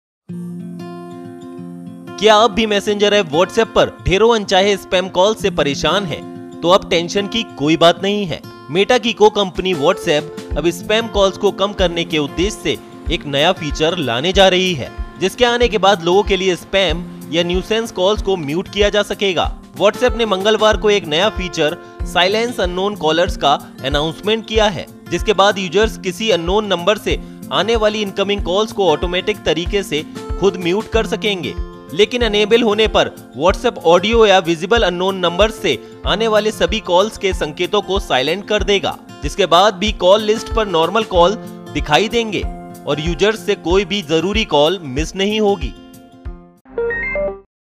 व्हाट्सऐप पर स्पैम कॉल्स से मिलेगी छुट्टी, यूजर्स खुद इनकमिंग कॉल को कर सकेंगे म्यूट
व्हाट्सऐप स्पैम कॉल्स से बचने के लिए Silence Unknown Callers नाम का नया फीचर लाने जा रहा है. जिसके आने के बाद यूजर्स किसी अननोन नंबर से आने वाली इनकमिंग कॉल को ऑटोमैटिक तरीके से खुद म्यूट कर सकेंगे.